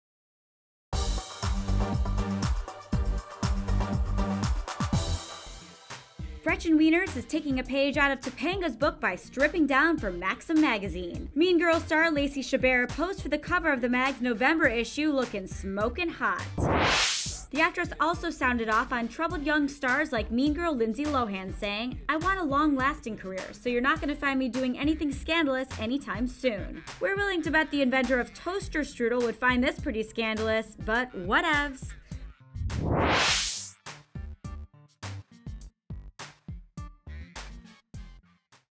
Lacey Chabert Covers Maxim
Gretchen Weiners is taking a page out of Topanga's book by stripping down for Maxim Magazine. Mean Girls star Lacey Chabert posed for the cover of the mag's November issue lookin' smokin' hot. The actress also sounded off on troubled young stars like Lindsay Lohan saying, "I want a long lasting career, so you're not gonna find me doing anything scandalous anytime soon." We're willing to be the inventor of toaster strudel would find this scandalous-- but whatevs!